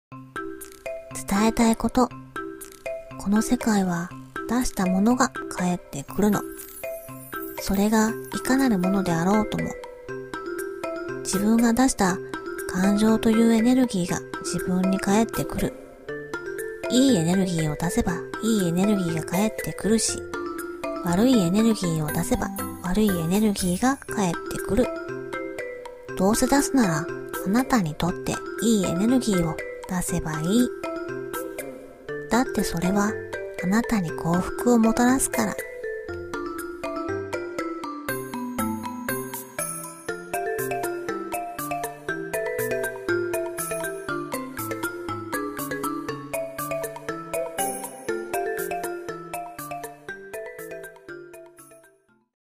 【癒し系スピリチュアルメッセージ】Supiritual message relax voice『ブーメラン』ＣＣ：Eng、Cha 、Kor sab
私達は、日々成長しています。そして日々様々な学びをしています。私達は、この世界に学びという修行をしに来ています。全ての出来事は、全て宇宙からのギフトなんです。良くも悪くも全て宇宙からのギフトなんです。どんな経験も学びに繋がっています。

ブログ（テラの宝物❤聖なるパワーストーン）